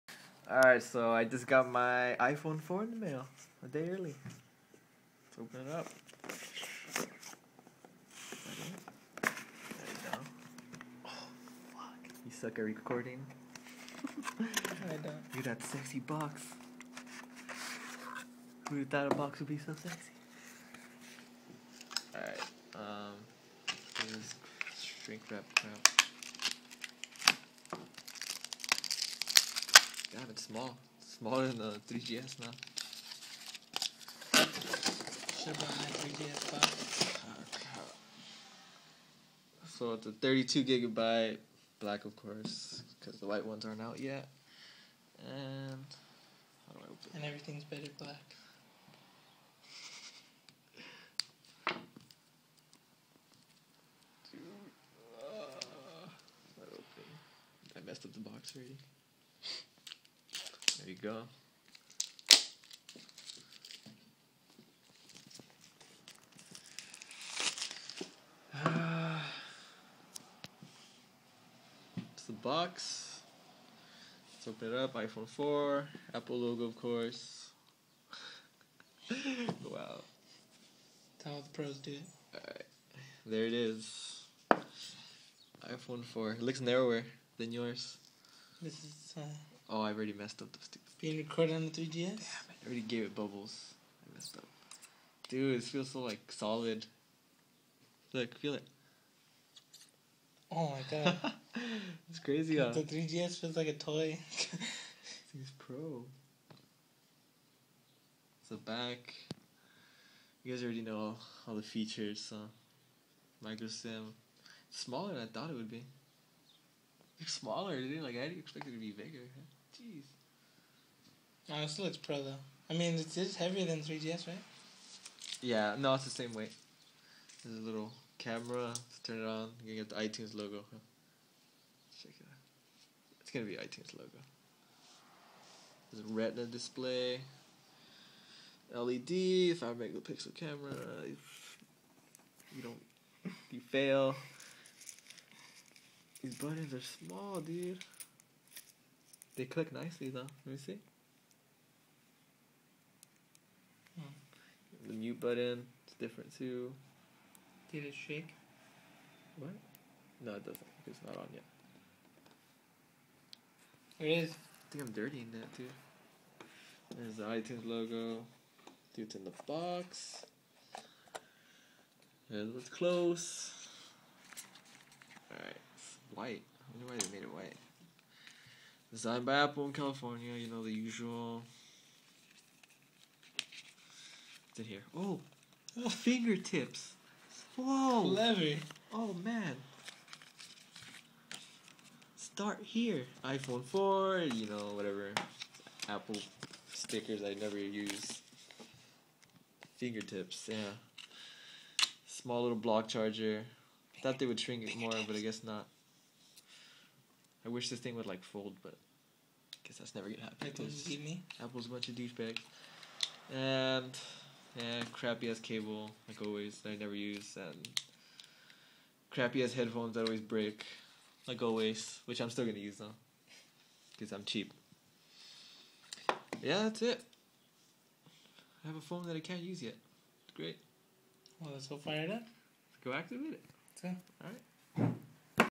IPhone 4 unboxing
my friend Got the iPhone 4 a day early and yah it's an unboxing